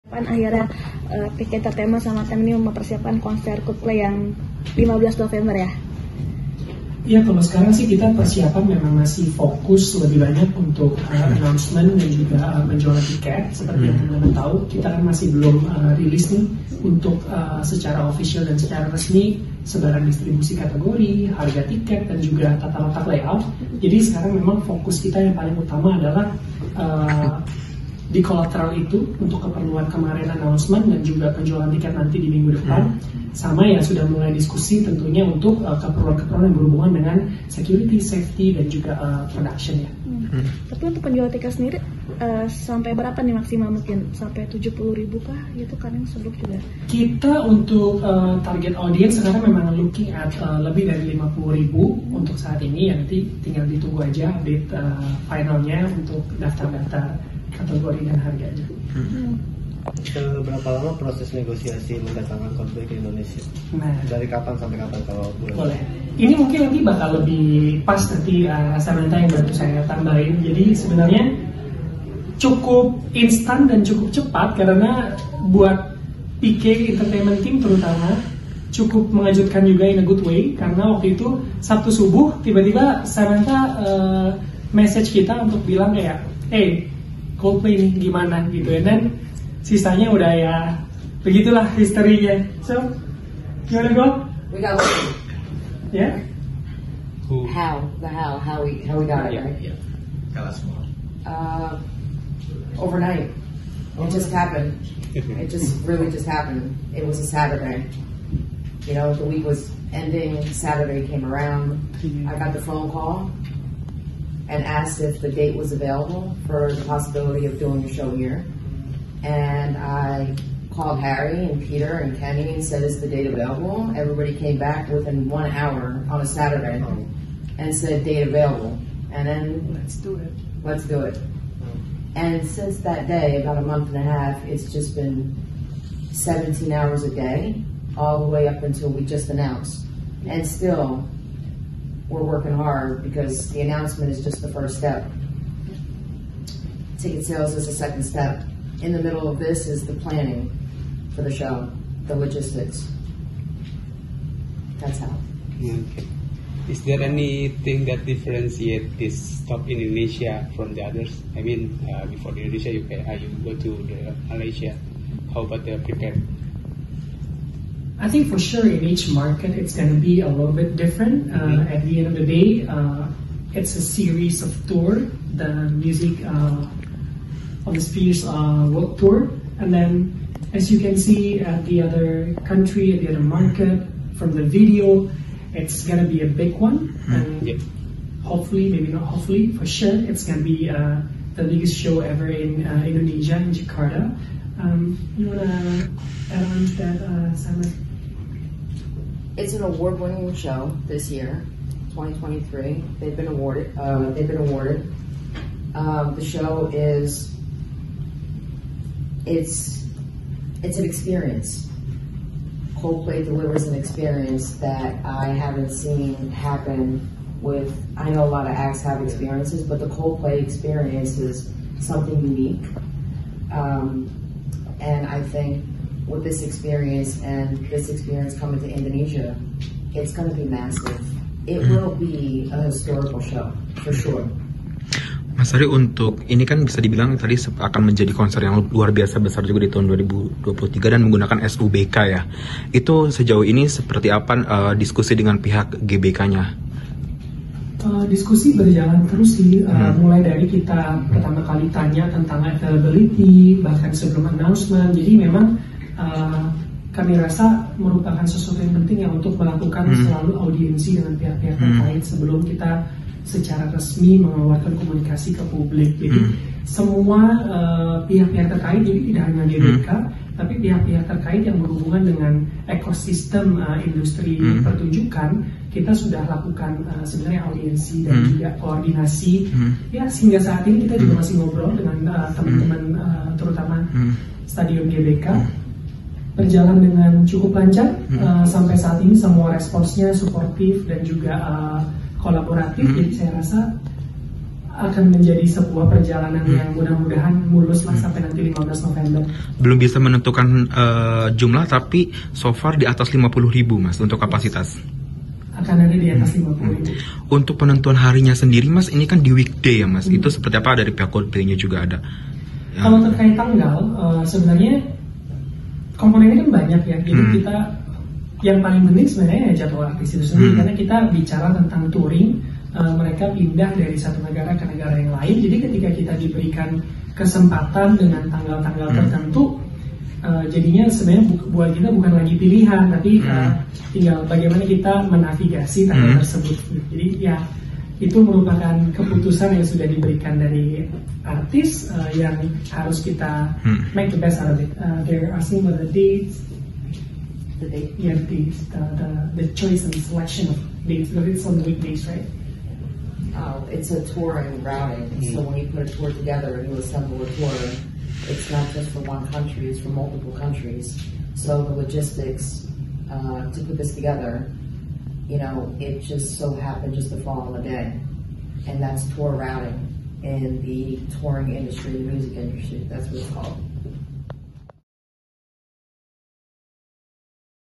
Promotor Bahas Opening Act Konser Coldplay di Indonesia
VIVA - Pihak promotor membahas opening act atau penampil pembuka dalam konser Coldplay di Jakarta nanti. Seperti yang sudah diketahui, Coldplay akan menggelar konser bertajuk "Coldplay Music of The Spheres World Tour" di Stadion Utama Gelora Bung Karno (SUGBK), pada 15 November 2023.

Pada saat ditemui awak media, pihak promotor dari PK Entertainment dan TEM Presents membahas beberapa hal terkait konser Coldplay di Indonesia nanti. Salah satunya adalah mengenai opening act. Harry Sudarma selaku Co-founder & COO PK Entertainment menjelaskan bahwa opening act kemungkinan besar akan ada di konser Coldplay nanti. (Aiz Budhi)